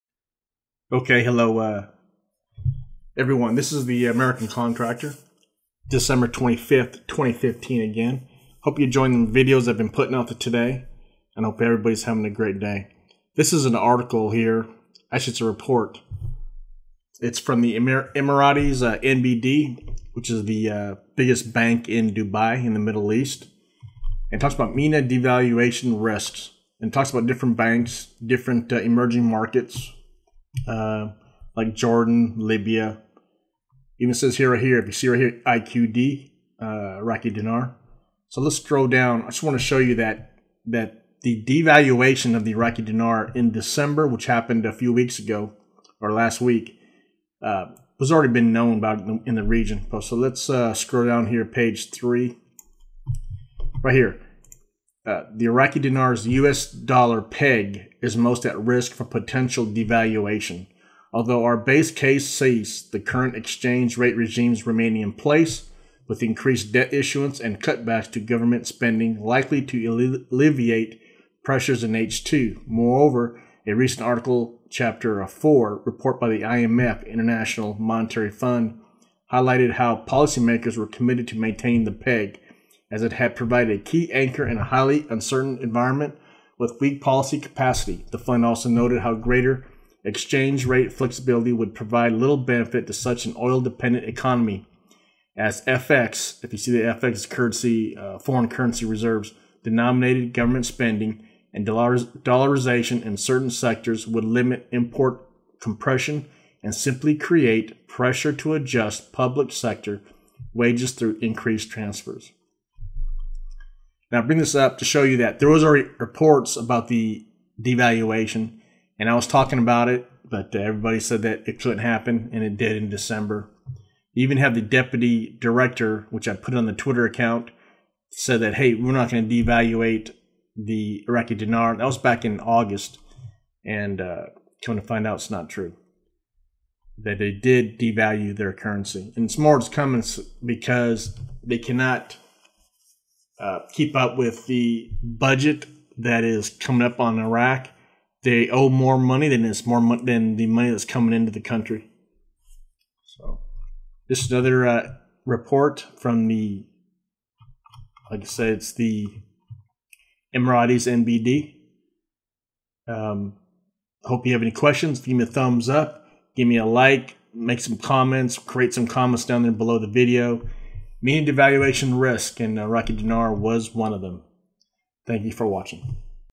Iraqi Dinar devaluation Says Emirates NBD Report
Middle Eastern banks were even talking about the upcoming devaluation of the Iraqi dinar.